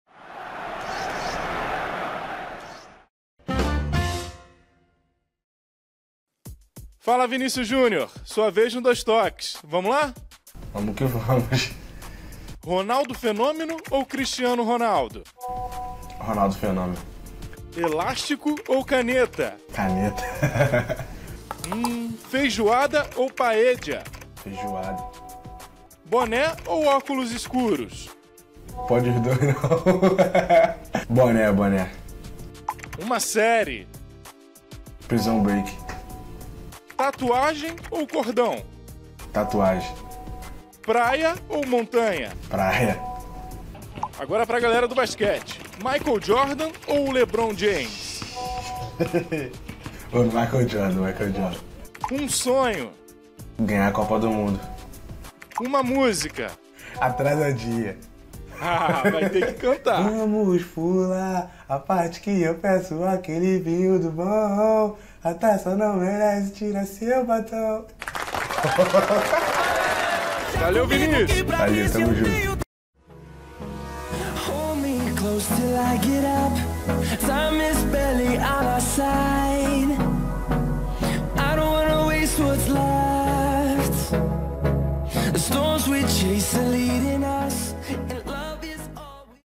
El test sobre la vida personal de Vinicius: "¿Ronaldo Nazario o Cristiano?" ⚽ REVIEW ⚽ 2018
🔸 Vinicius Jr. se sometió en Brasil a un test sobre su vida personal. Eligió a su favorito entre Cristiano y Ronaldo Nazario, entre LeBron y Jordan...